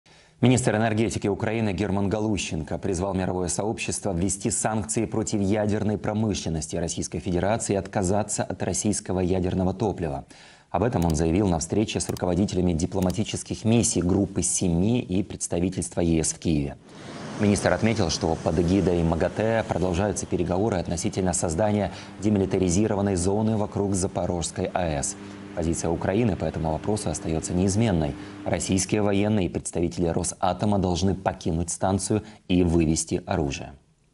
Украина призывает мир реагировать на ядерный шантаж рф
Украина призывает мир ввести санкции против ядерной промышленности рф и вывести войска с территории ЗАЭС.